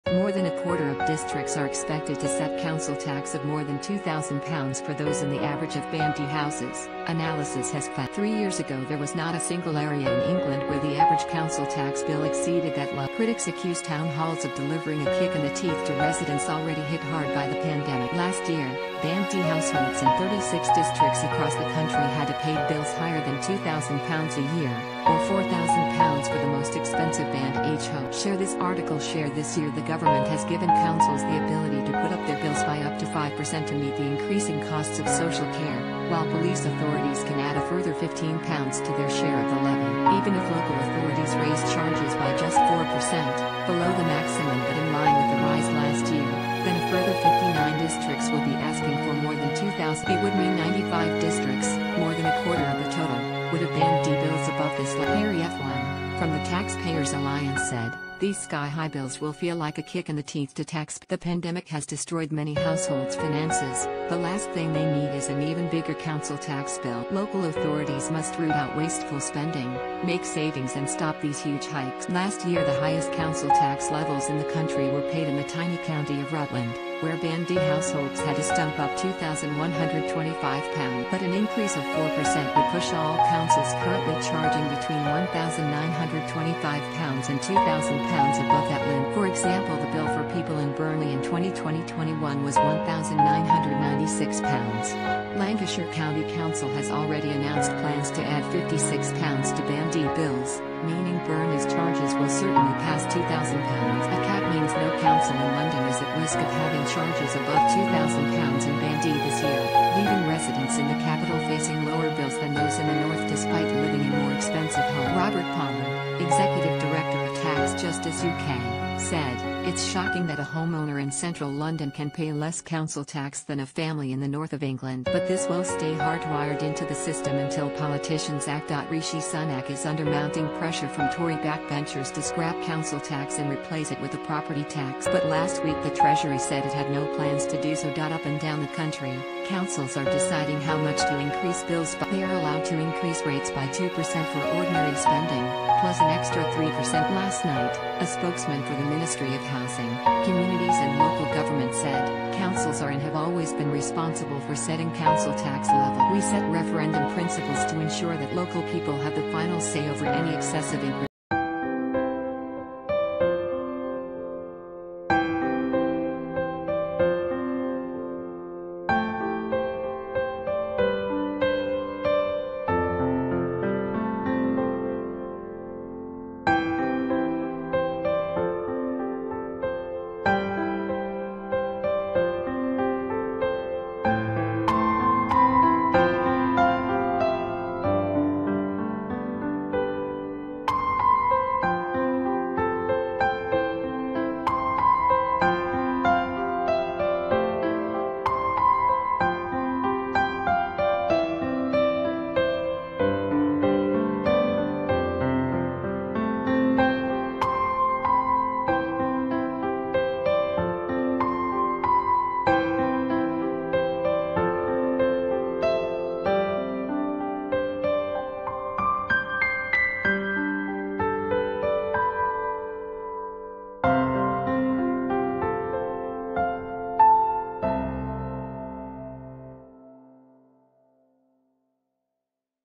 Millions will be battered by £2,000 council tax in 25% of areas
.More than a quarter of districts are expected to set council tax of more than £2,000 for those in the average of Band D houses, analysis has found.Three years ago there was not a single area in England where the average council tax bill exceeded that level.  Critics accused town halls of delivering a 'kick in the teeth' to residents already hit hard by the pandemic.Last year, Band D households in 36 districts across the country had to pay bills higher than £2,000 a year, or £4,000 for the most expensive Band H homes.   Share this article   Share   This year the Government has given councils the ability to put up their bills by up to 5 per cent to meet the increasing costs of social care, while police authorities can add a further £15 to their share of the levy.Even if local authorities raise charges by just 4 per cent – below the maximum but in line with the rise last year – then a further 59 districts will be asking for more than £2,000.   It would mean 95 districts – more than a quarter of the total – would have Band D bills above this level. Harry Fone, from the TaxPayers' Alliance said: 'These sky-high bills will feel like a kick in the teeth to taxpayers.'The pandemic has destroyed many households' finances – the last thing they need is an even bigger council tax bill. Local authorities must root out wasteful spending, make savings and stop these huge hikes.'Last year the highest council tax levels in the country were paid in the tiny county of Rutland, where Band D households had to stump up £2,125. But an increase of 4 per cent would push all councils currently charging between £1,925 and £2,000 above that limit.   For example the bill for people in Burnley in 2020/21 was £1,996. Lancashire County Council has already announced plans to add £56 to Band D bills – meaning Burnley's charges will certainly pass £2,000.A cap means no council in London is at risk of having charges above £2,000 in Band D this year, leaving residents in the capital facing lower bills than those in the North despite living in more expensive homes.Robert Palmer, executive director of Tax Justice UK, said: 'It's shocking that a home owner in central London can pay less council tax than a family in the North of England.   'But this will stay hard-wired into the system until politicians act.'Rishi Sunak is under mounting pressure from Tory backbenchers to scrap council tax and replace it with a property tax. But last week the Treasury said it had no plans to do so.Up and down the country, councils are deciding how much to increase bills by.They are allowed to increase rates by 2 per cent for ordinary spending, plus an extra 3 per cent for social care.Last night, a spokesman for the Ministry of Housing, Communities and Local Government said: 'Councils are and have always been responsible for setting council tax levels. We set referendum principles to ensure that local people have the final say over any excessive increases.'